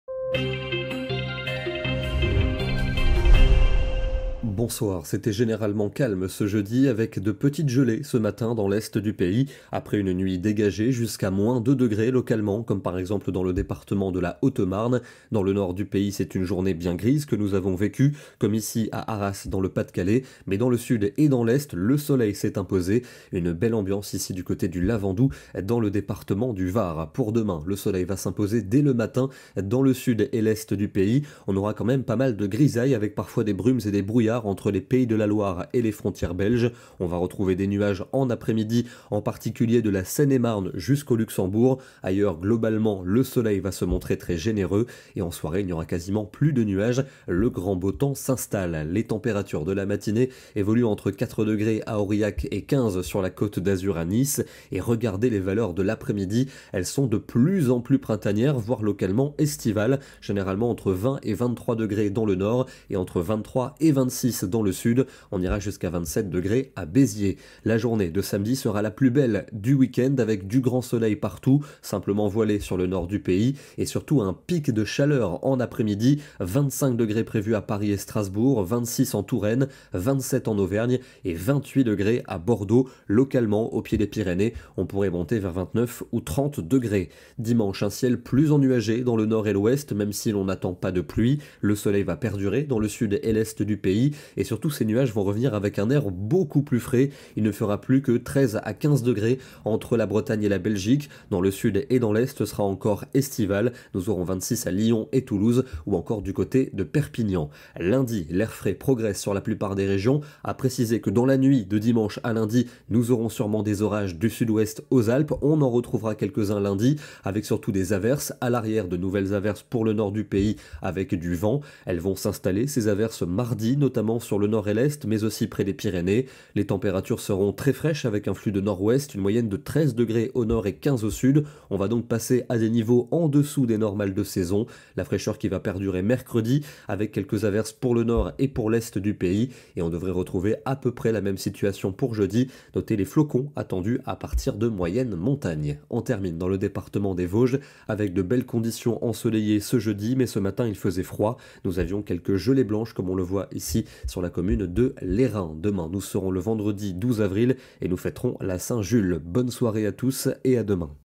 Météo du Vendredi 12 au Jeudi 18 Avril 2024
Prévisions météo du Vendredi 12 au Jeudi 18 Avril 2024
Résumé : Très printanier ce vendredi. Coup de chaud samedi puis net rafraîchissement pour la mi-avril.

🌤 Ce bulletin météo vous est présenté par Alexandre Slowik.